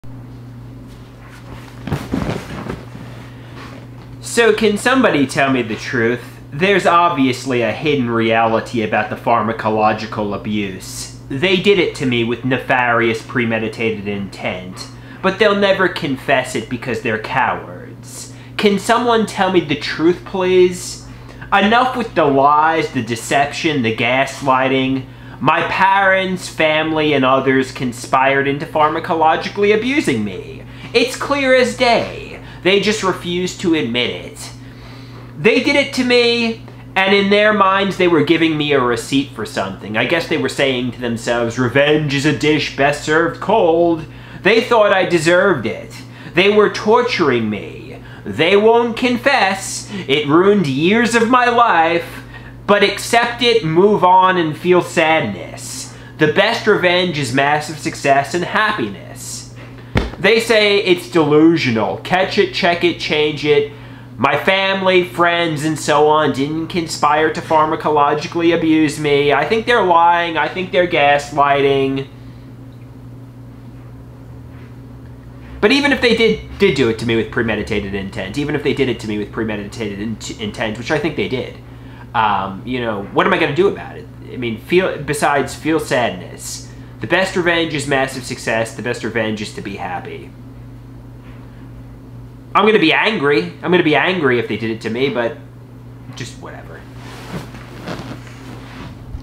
They Did It (08 06 2024)
They Did It.

Can somebody tell me the truth?

There's obviously a hidden reality about the pharmacological abuse. They did it to me with NEFARIOUS PREMEDITATED INTENT. But they'll never confess it because they're cowards

CAN SOMEONE TELL ME THE FUCKING TRUTH, PLEASE?

Enough with the lies, deception, and gaslighting. My parents, family, and others conspired into pharmacologically abusing me. It's clear as day. They just refuse to admit it.

They did it to me. In their minds, they were giving me a "receipt" for something. I guess they were saying to themselves: "revenge is a dish best served cold." They thought I deserved it. They were torturing me. They won't confess. It ruined years of my life. But accept it, move on, and feel sadness. The best revenge is massive success and happiness.

They say "it's delusional." Catch it, check it, change it. My family, friends, and so on didn't conspire to pharmacologically abuse me.

I think they're lying. I think they're gaslighting. But even if they did do it to me with premeditated intent - which I think they did - what am I going to do about it besides feel sadness? The best revenge is massive success and to be happy. I'm going to be angry if they did it to me.

Just whatever.